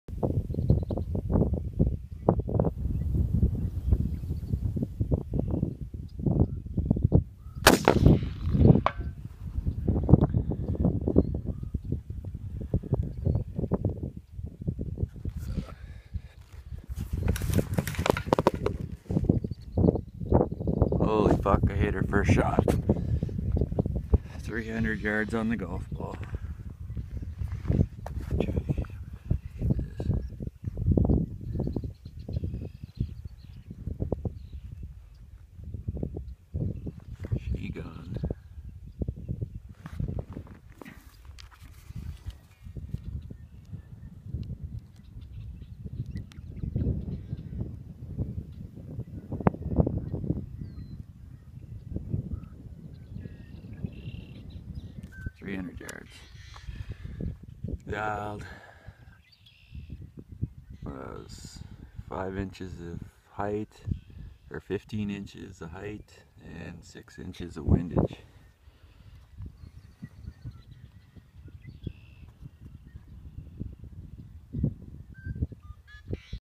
scott Chychul shooting golf ball 300 yards
Shooting a golf ball at 300 yards with a Remington 700 AAC-SD 20" barrel in a 20-45 km side wind off hand while filming through phone.